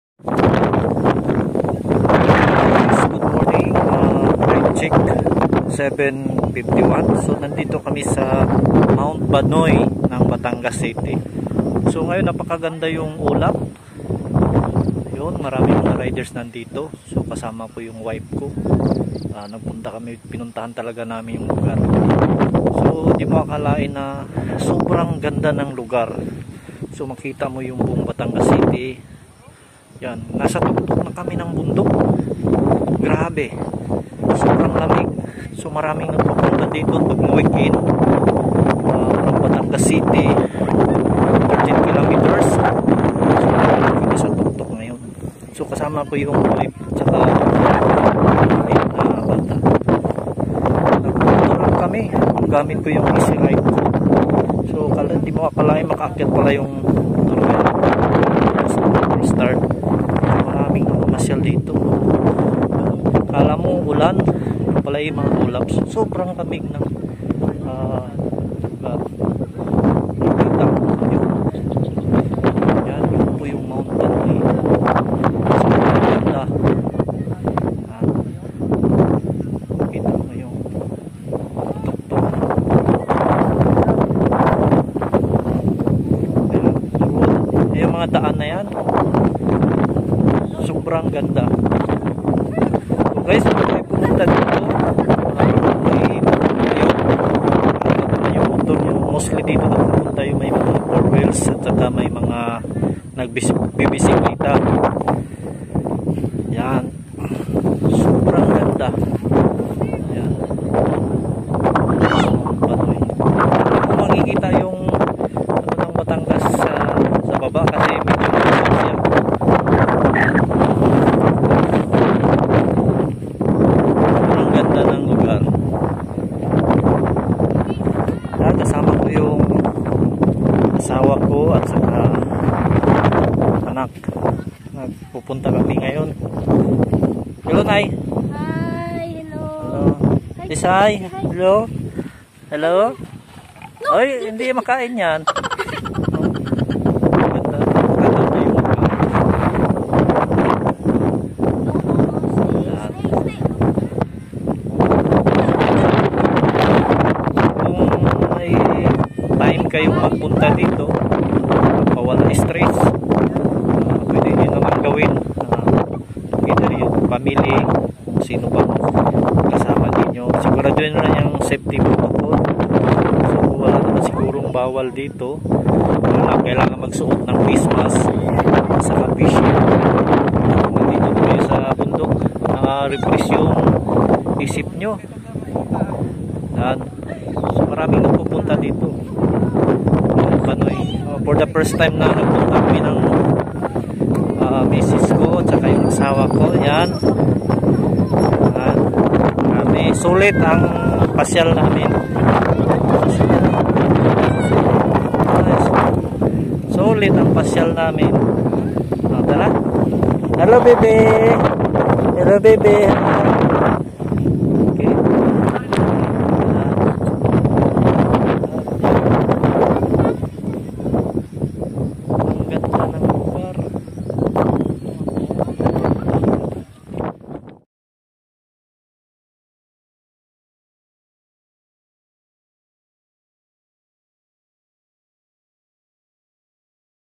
"THE MOUNT BANOY"
Mount Banoy is a summit in Batangas and has an elevation of 960 metres. Mount Banoy is situated south of Talumpoc, west of Mount Liguaye